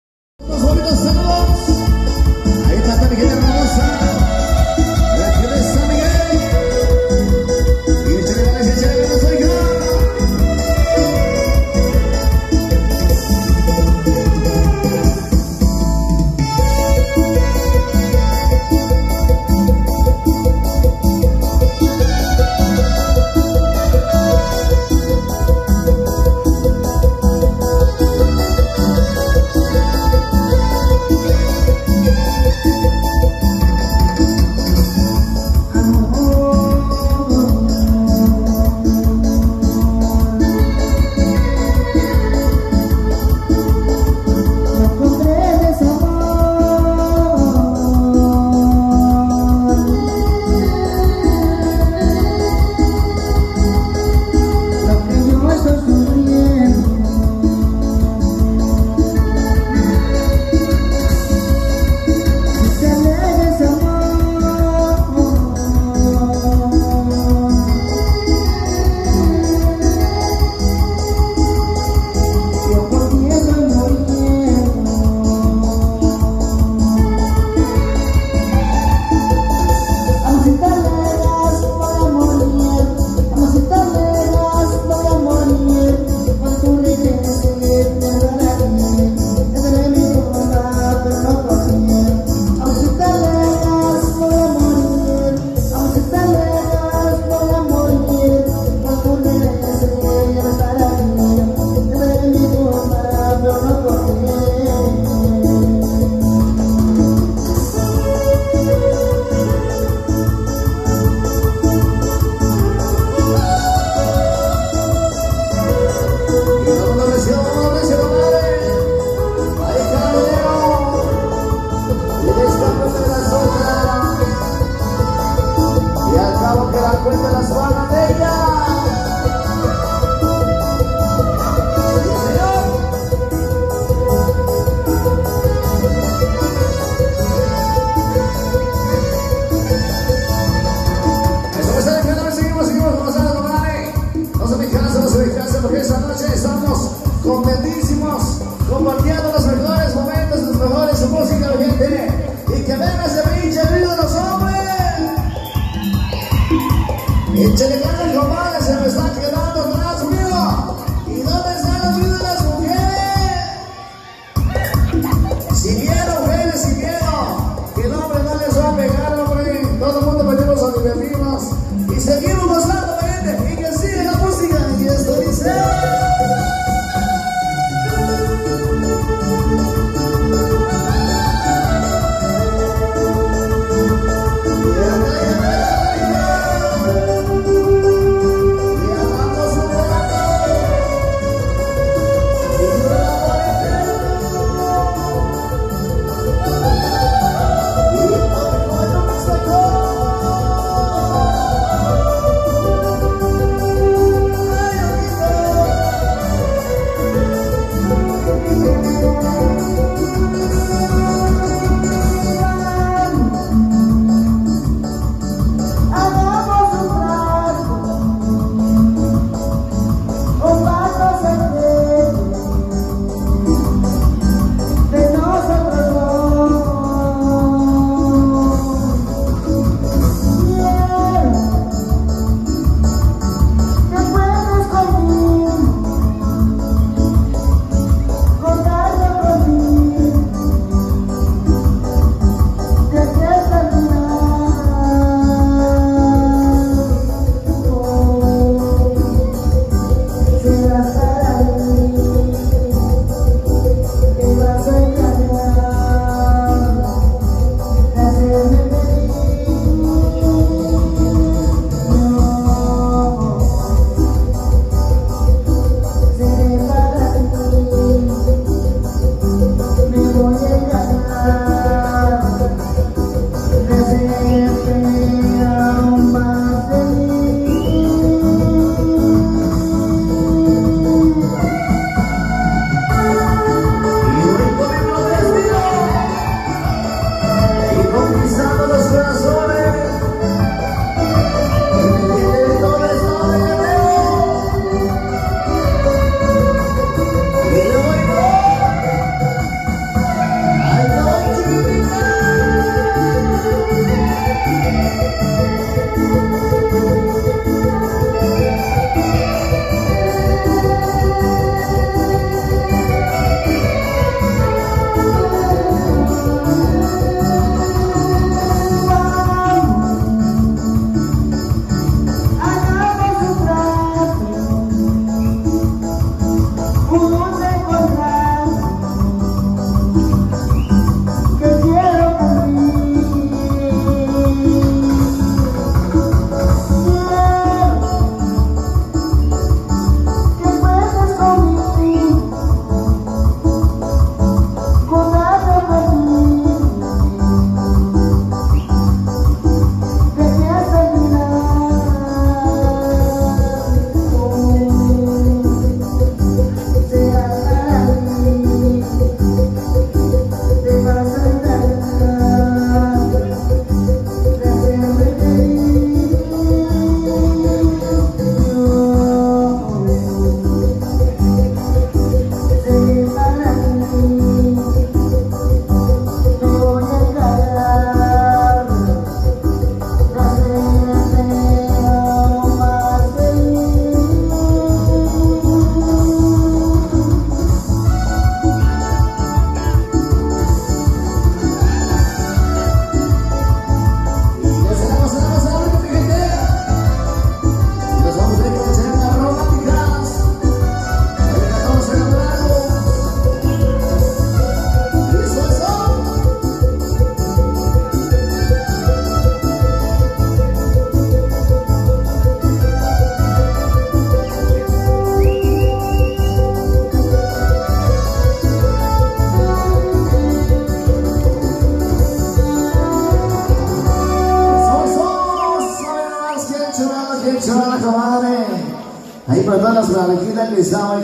Juventud Mixteco, en San MiguelitoTlacoapa, 14 de diciembre 2023
Gran baile en San MiguelitoTlacoapa, 14 de diciembre 2023 con el grupo Organo Juventud Mixteco